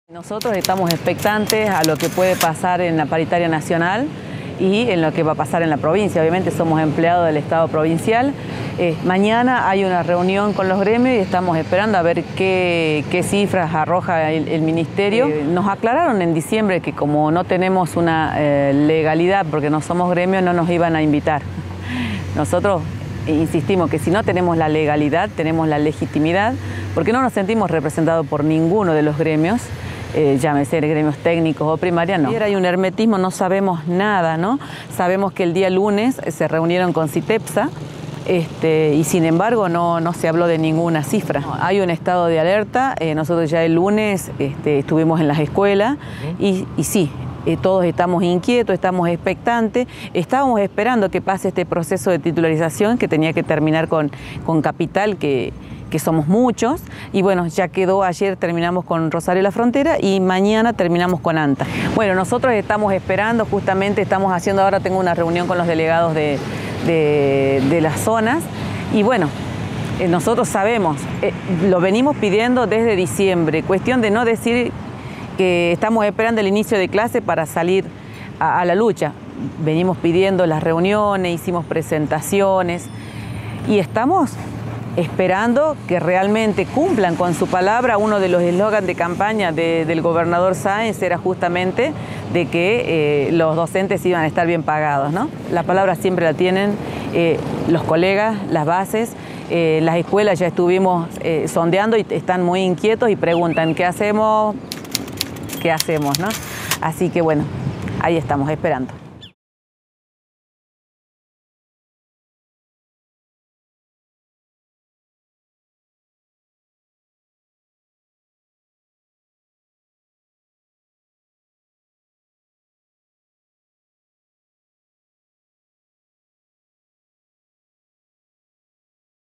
Salta: Autoconvocados en estado de alerta
Expectativa por la reunión entre los gremios y el gobierno. No descartaron medidas de fuerza.

A diez días de la fecha pautada para el inicio del ciclo lectivo 2020 en Salta, los docentes de nuestra provincia siguen sin un acuerdo salarial que garantice el regreso a las aulas el próximo 2 de marzo.